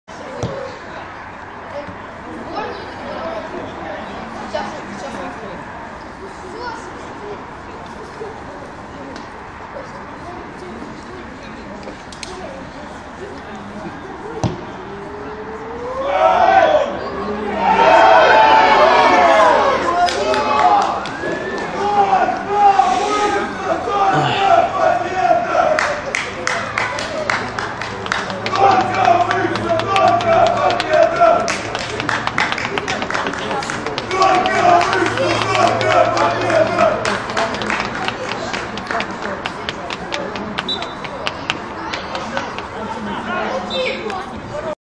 Подолье - Металлург Выкса - 5:1 (2:0). Гол Максима Процерова
Второй дивизион, зона Центр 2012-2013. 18 тур. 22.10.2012.